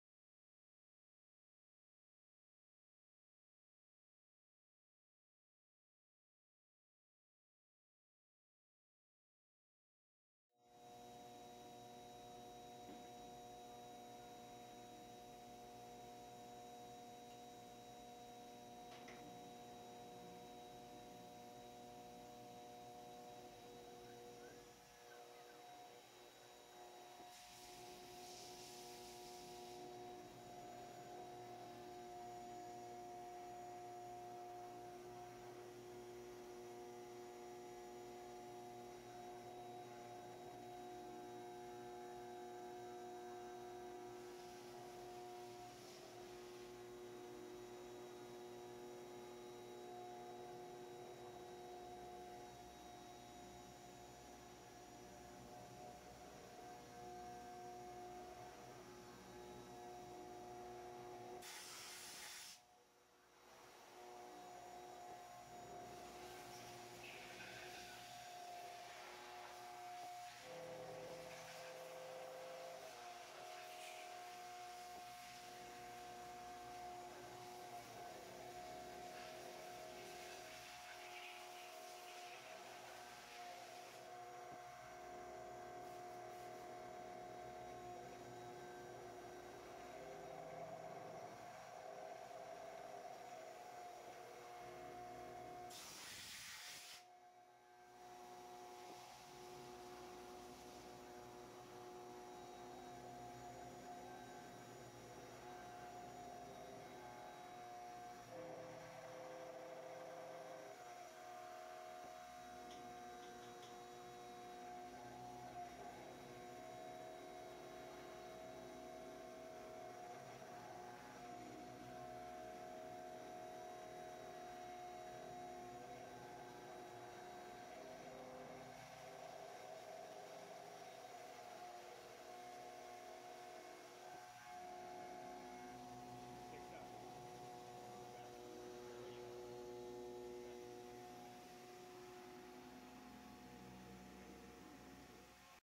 HONING H312 mill
Cutting a pocket with the new H312 desktop mill.
3/8 3 flute carbide end mill, 5700 rpm, with a depth of cut of .03 in 6160 aluminum